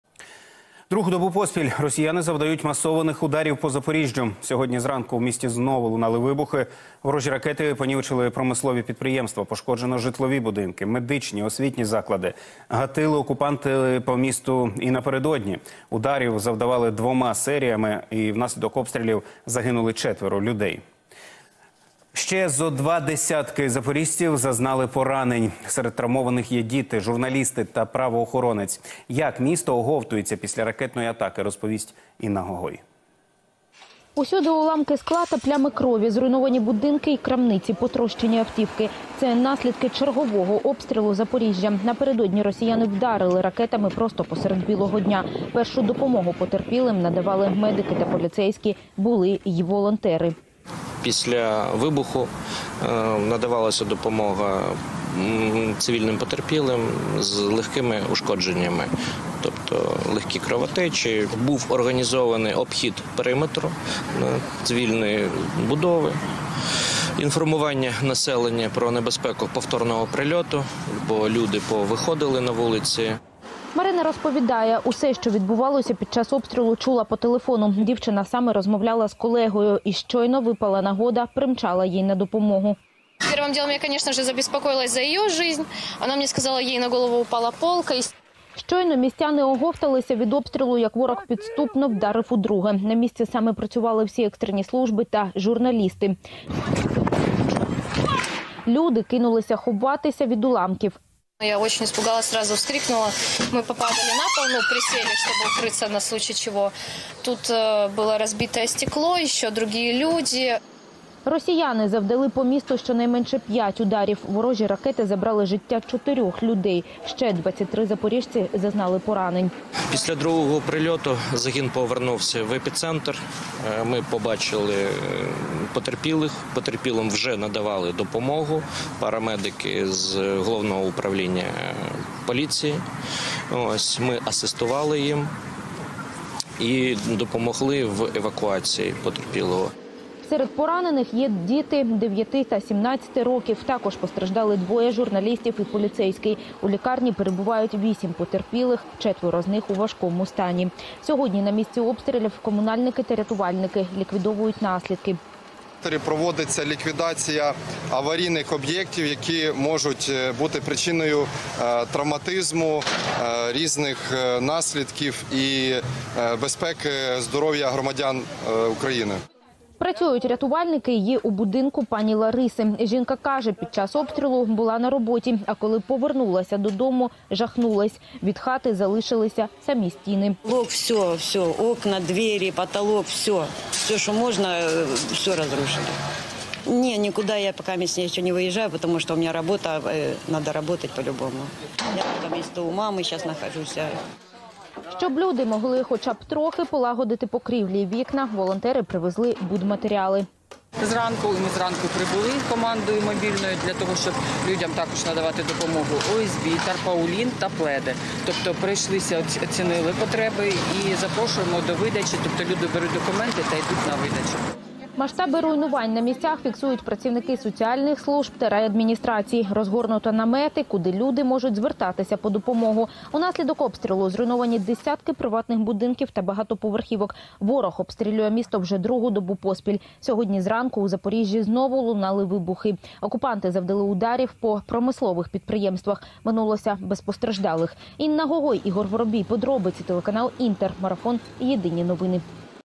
РАКЕТНИЙ УДАР по Запоріжжю: як оговтуються містяни
Другу добу поспіль росіяни завдають масованих ударів по Запоріжжю. Сьогодні зранку у місті знову лунали вибухи. Ворожі ракети понівечили промислові підприємства. Пошкоджено житлові будинки, медичні та освітні заклади. Гатили окупанти по місту й напередодні. Удари завдавали двома серіями. Внаслідок обстрілів загинули четверо людей. Ще зо два десятки запоріжців зазнали поранень. Серед травмованих - діти, журналісти та правоохоронець. Як місто оговтується після ракетної атаки - розповість Інна Гогой.